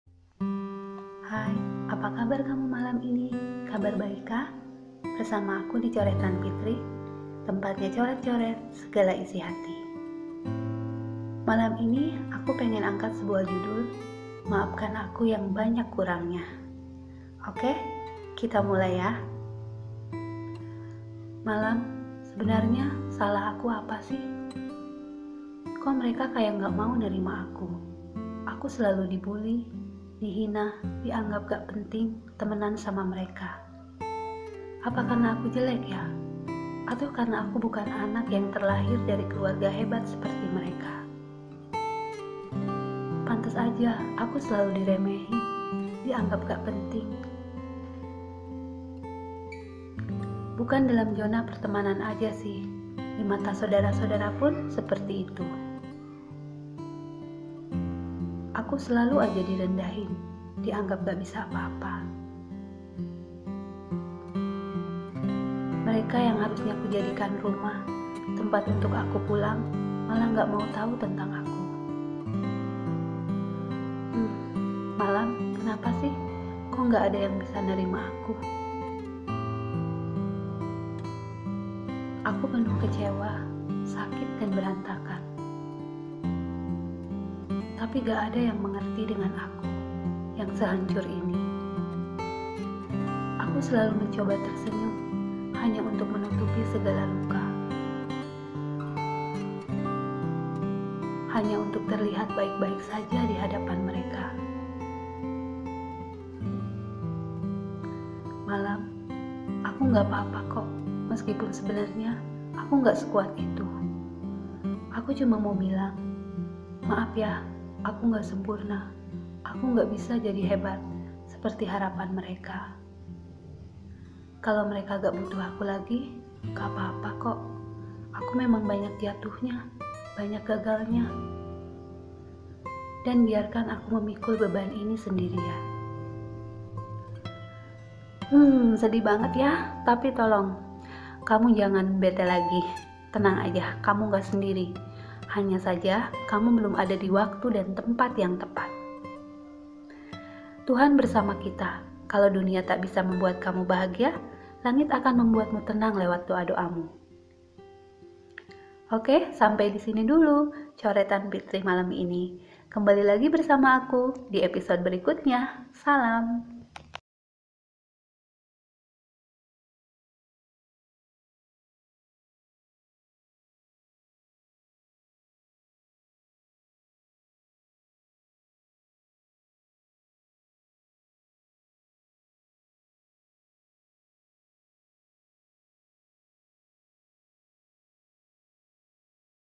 Maafkan aku yang banyak kurangnya
Maafkan aku yang banyak kurangnya adalah podcast monolog tentang kata hati

Katahati#galaustory